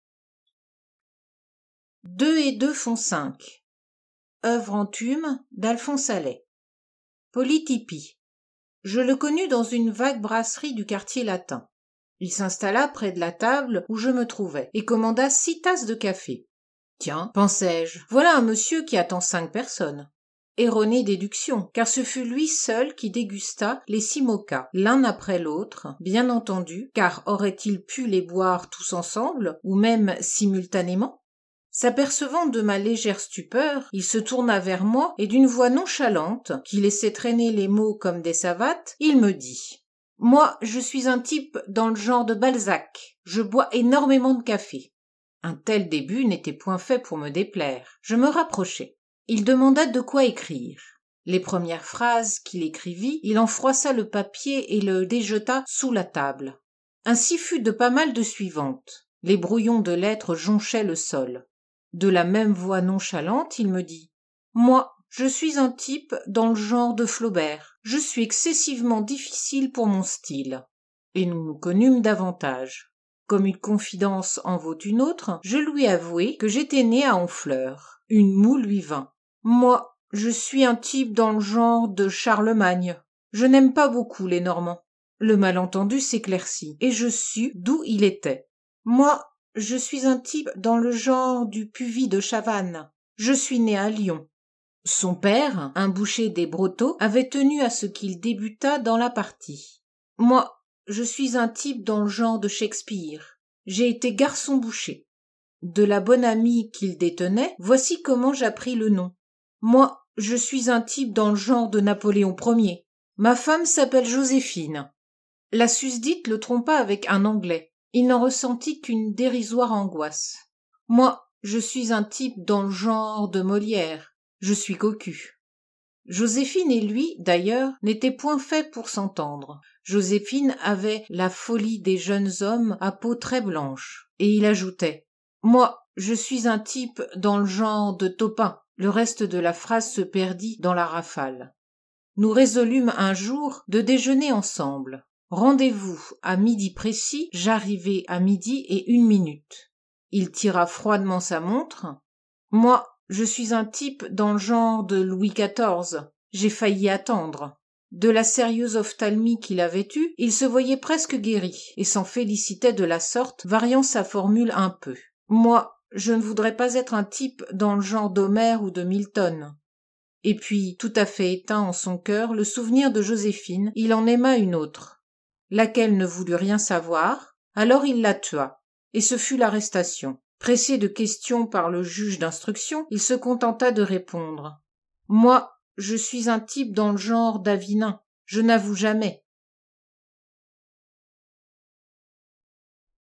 Deux et deux font cinq - Alphonse Allais
Dans ce livre sont rassemblés des dizaines d'articles humoristiques publiés dans des journaux de la fin du XIXème siècle (première parution 1895) qui, malgré leur ancienneté, sont toujours bien agréables à lire. On s'aperçoit qu'Alphonse Allais avec son Captain Cap et tous ses personnages ridicules ou déjantés avait été un extraordinaire précurseur. Il annonçait déjà toute la lignée des auteurs absurdes, le dadaïsme, le surréalisme et même l'humour iconoclaste qui fleurira au XXème siècle. Comme il était également journaliste, il gardait un regard averti sur la société de son temps et savait mieux que personne ridiculiser les travers de ses contemporains.Les textes sont tous très courts, généralement une à trois pages, très incisifs et souvent sous la forme de dialogues percutants et hilarants avec des jeux de mots, des formules à l'emporte pièce d'une brillance fulgurante. Un aussi grand auteur que Courteline ou Feydeau avec le côté anarchiste et provocateur d'une totale modernité en plus. Il faut lire ou relire Alphonse Allais.